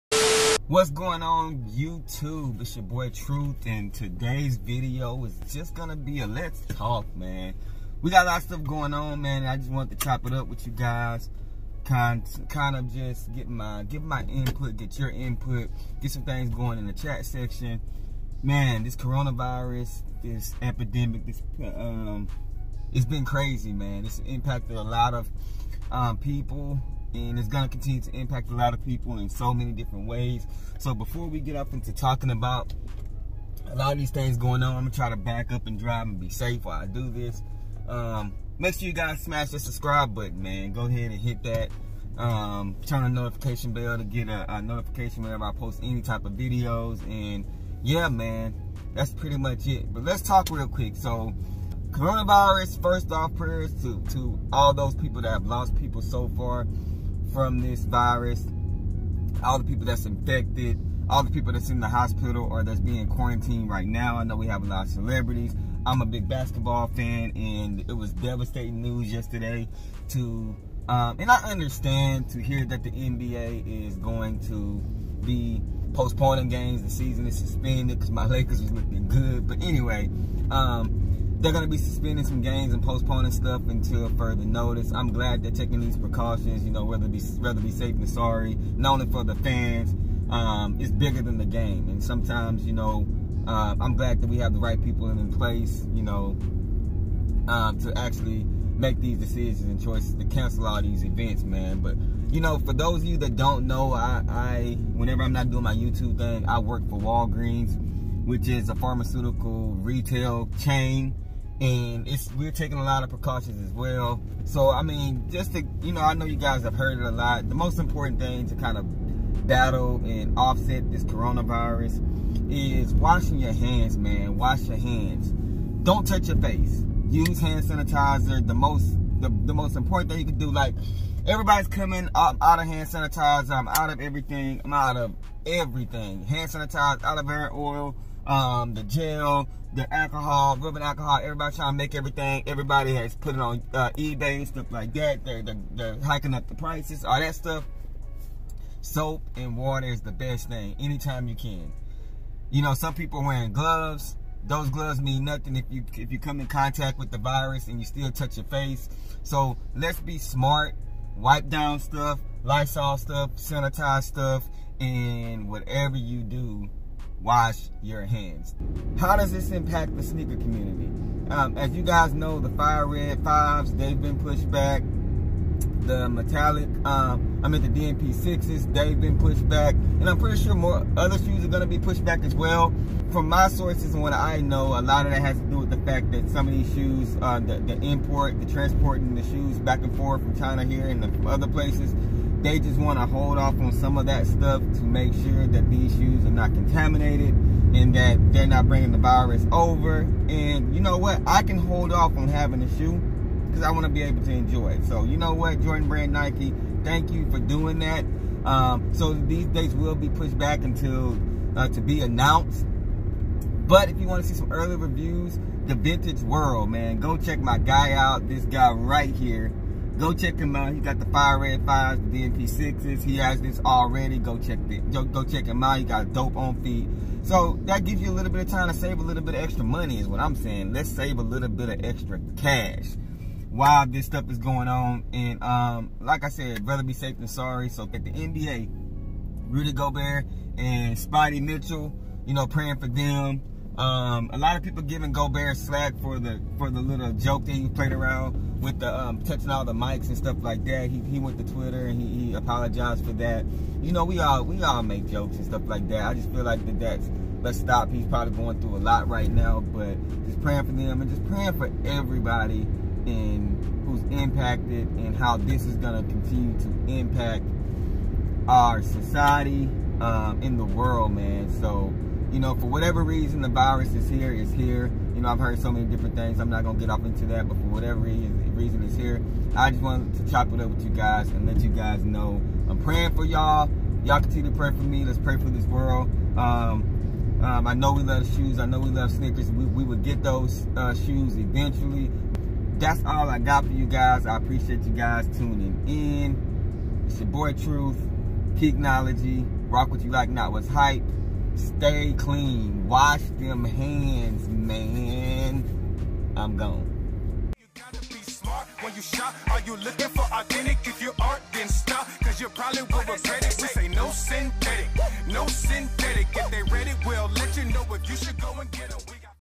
Chop It Up Wit Truth✊🏿Let's Talk💯
So much stuff going on this world. Sometimes you gotta just stop and talk it out. Chop it up with ya boy Truth💯